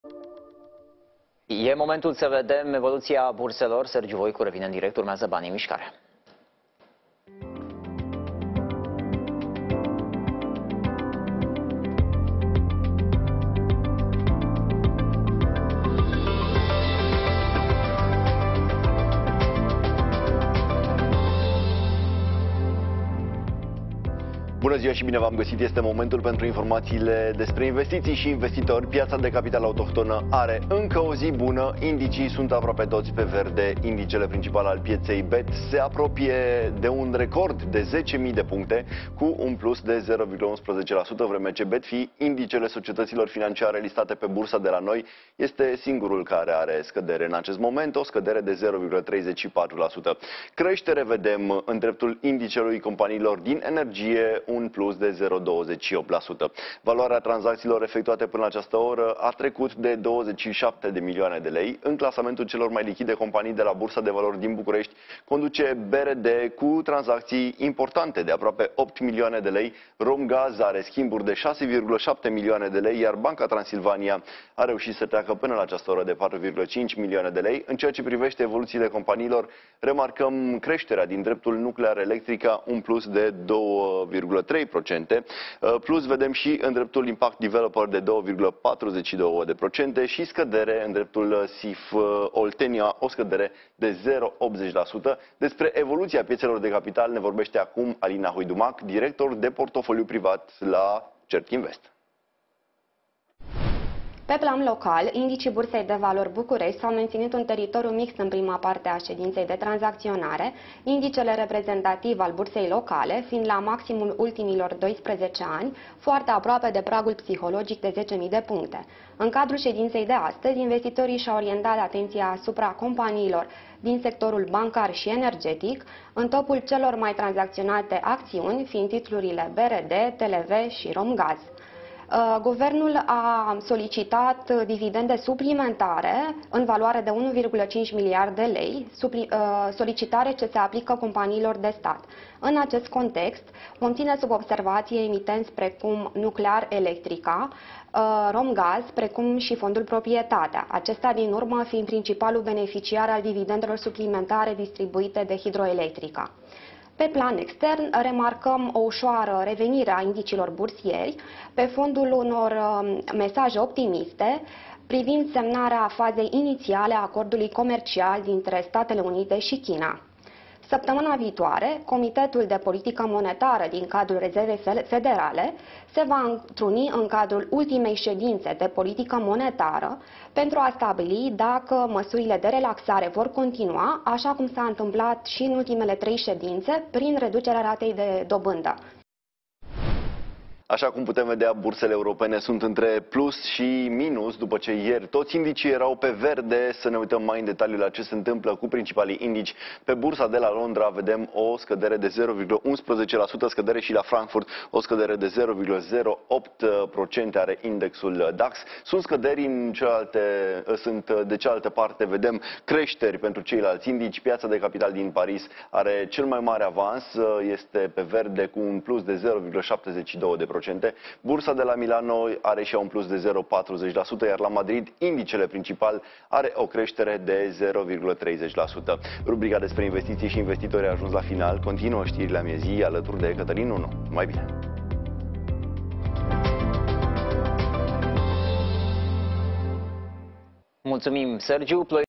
Banii in miscare cu Sergiu Voicu | Despre pietele de capital cu Alina Huidumac Certinvest
desper Bursa de Valori Bucuresti si evolutia ei ne vorbeste Alina Huidumac director de portofolii private la Certinvest